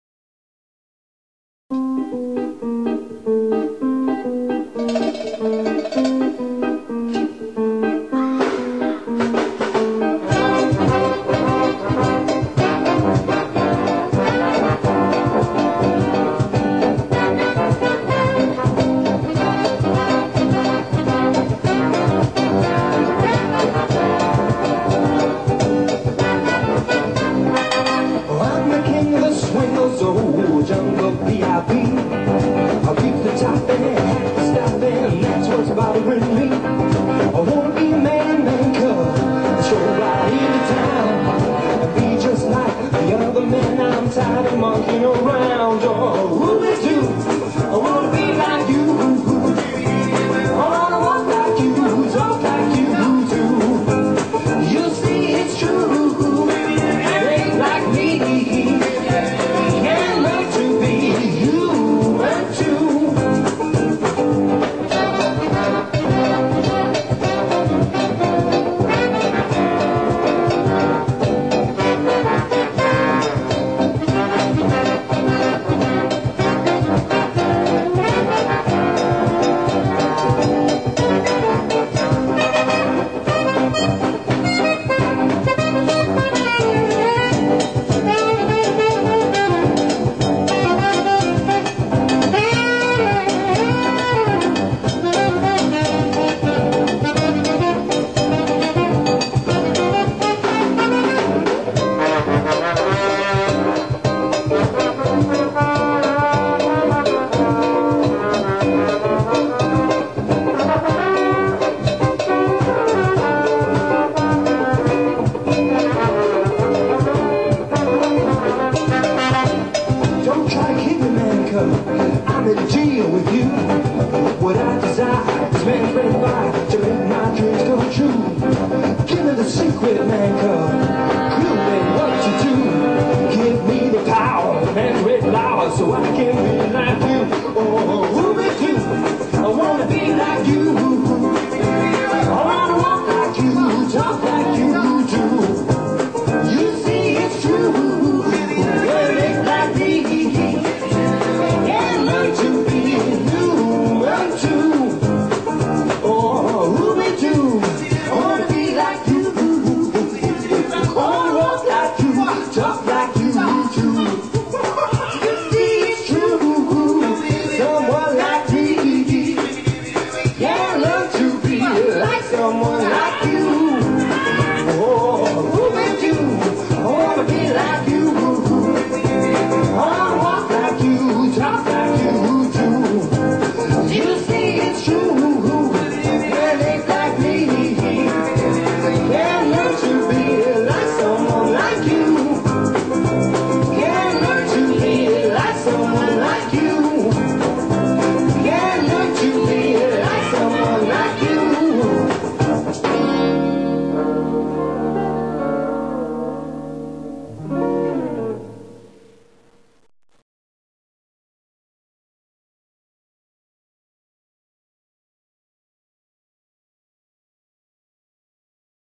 Other Act: The Top Bananas
The Top Bananas are a stiltwalking band of musicians, based in Brighton.

This band on stilts features a leaping monkey on bouncy stilts who plays an oil-can guitar, harmony vocals, a horn section, and a rhythm section. The band is all about fun and plays a selection of Jazz, Funk, Disco and Ska classics. Besides the Jungle Band there are also Black Tie and 70's Disco costumed versions available.  Bespoke themes and costumes possible with enough notice!